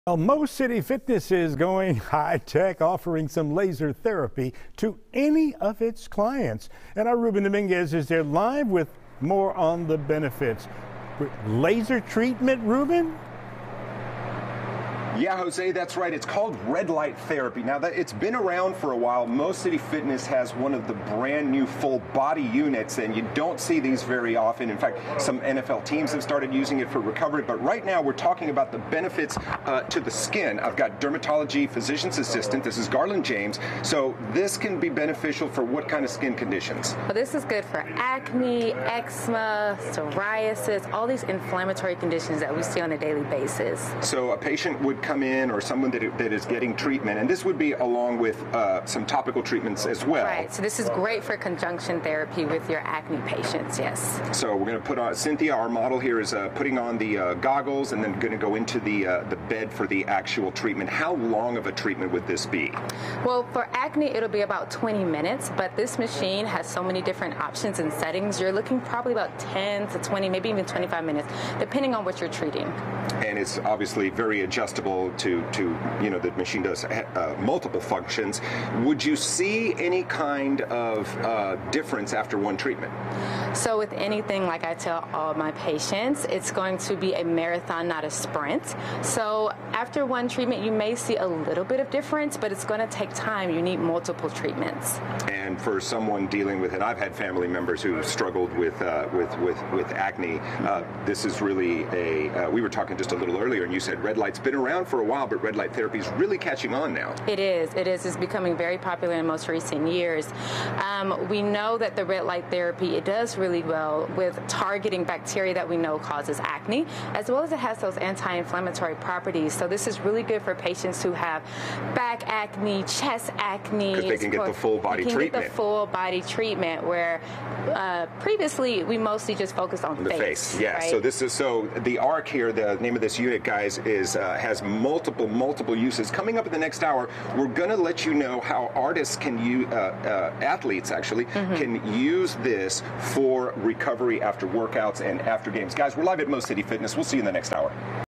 New laser therapy bed at Mo City Fitness
Mo City Fitness is going high-tech with laser therapy. Dermatology physician assistant Garland James discusses some of the benefits.